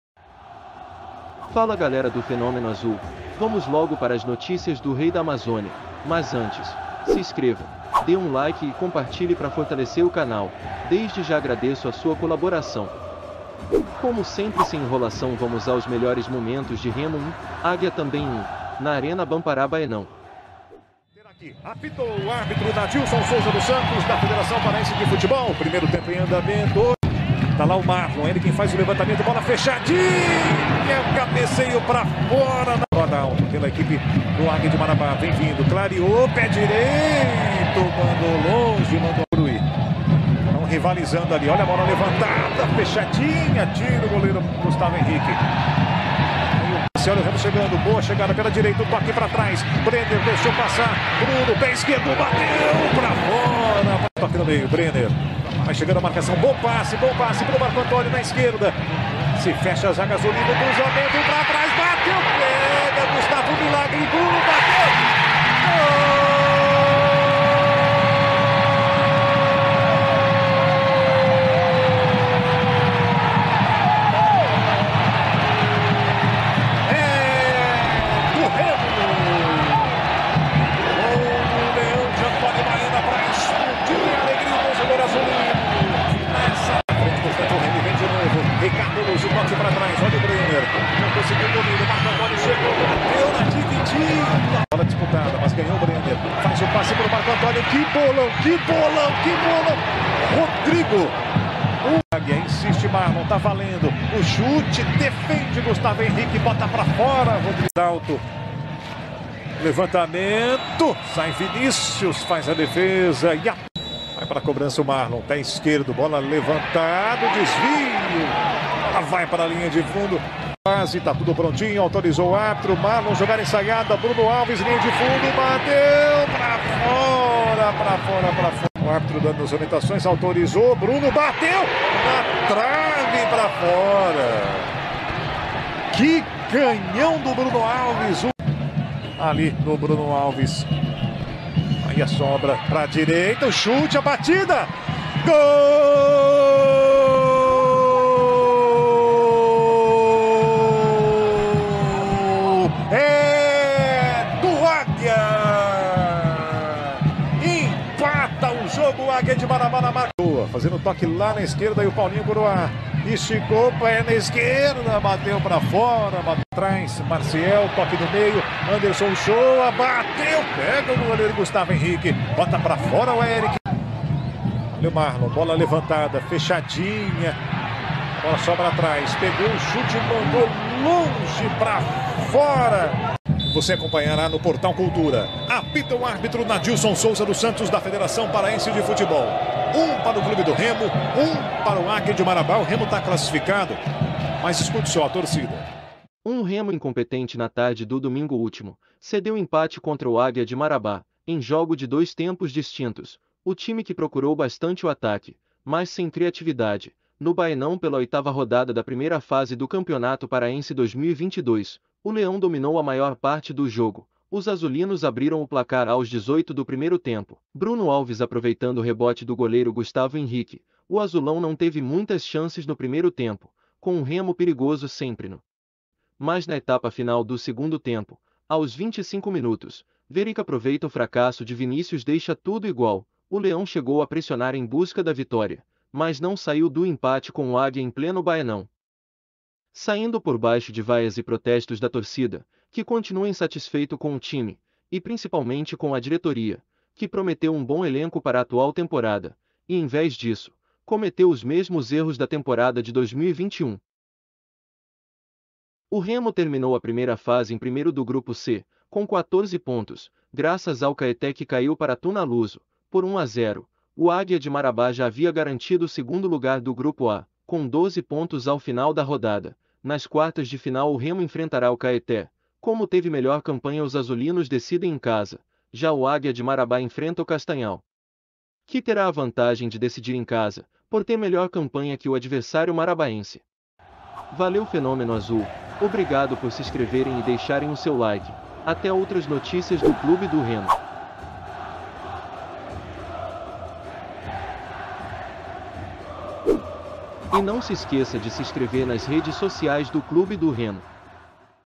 Melhores momentos Remo 1 x 1 Águia de Marabá e uma Rápida análise do que foi o jogo
Campeonato Paraense 2022. melhores momentos de Remo 1 x 1 Águia de marabá pela última Rodada da primeira Fase do Parazão 2022